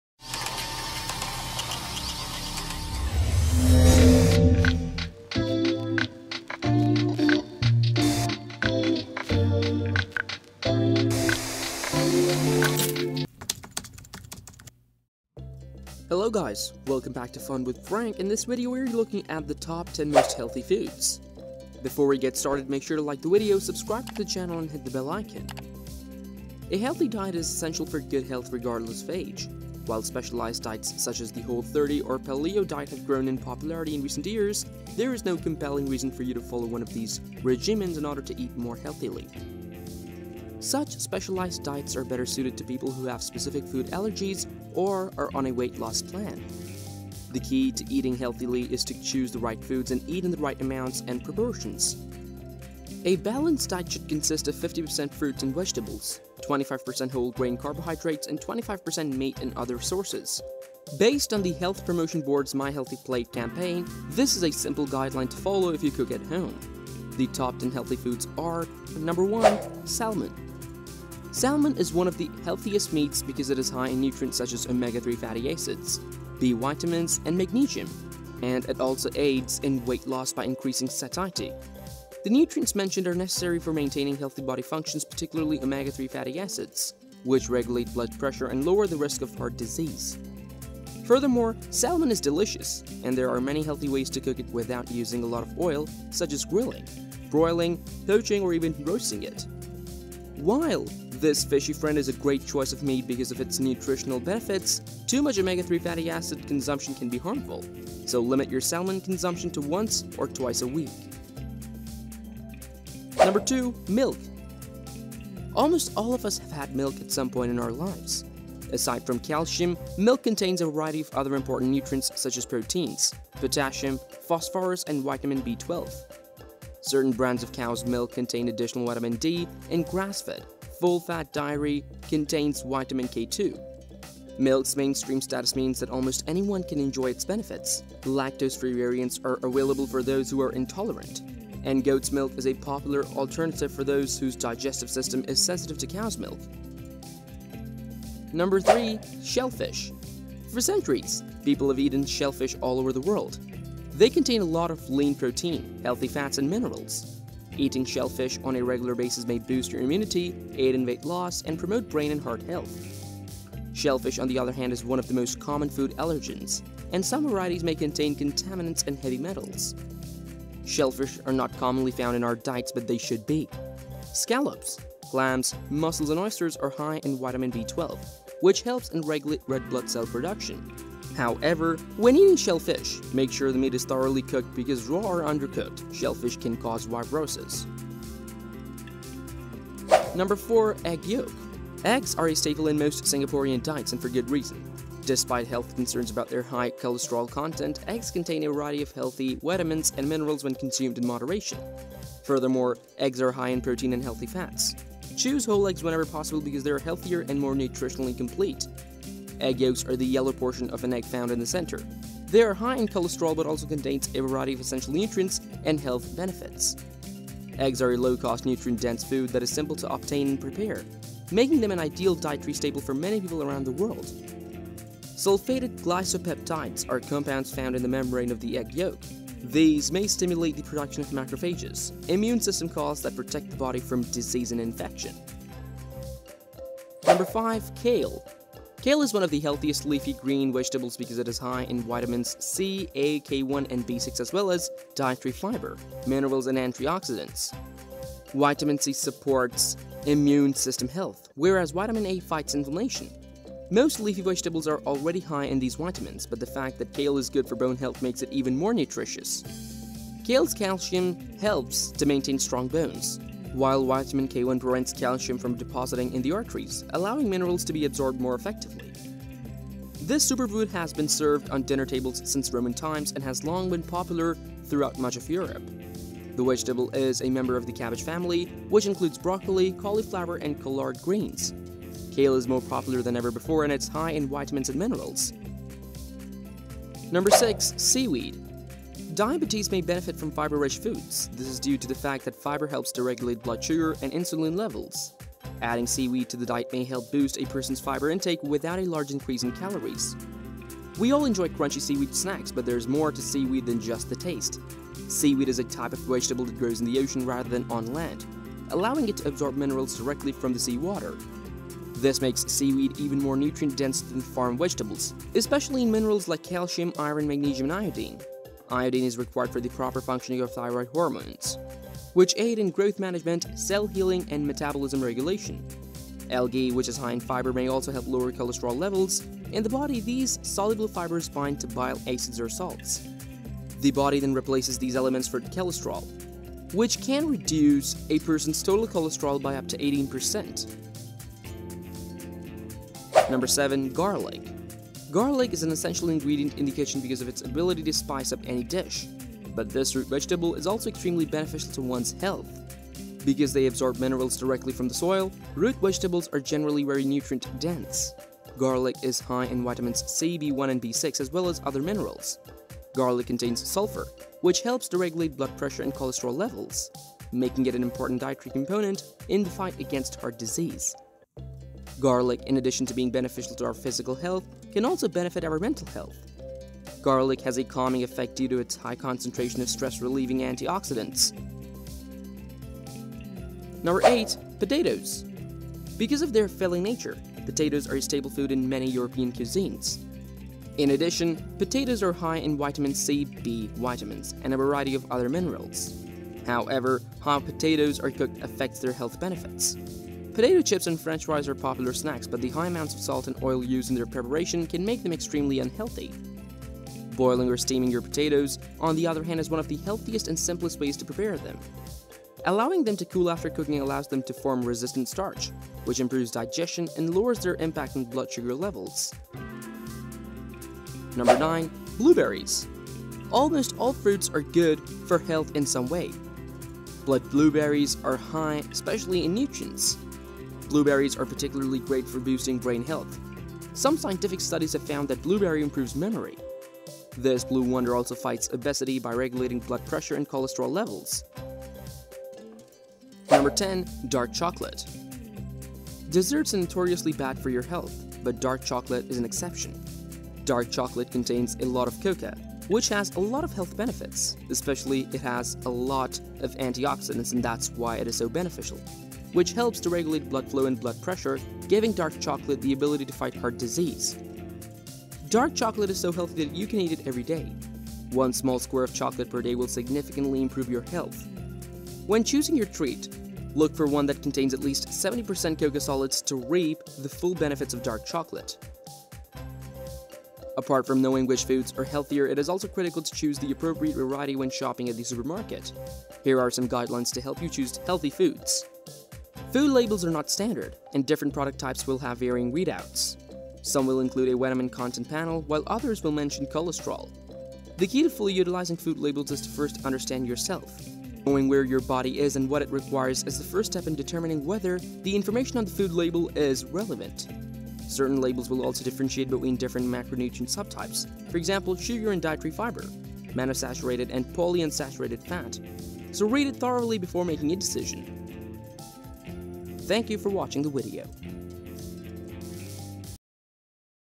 Top 10 Most Healthy Foods
A healthy lifestyle begins with a healthy diet. Come check out the most healthy foods to keep your body in tip top shape and heart health. You can enjoy eating healthy foods and feel better for yourself. Give it a try for a month and see how you feel with more energy!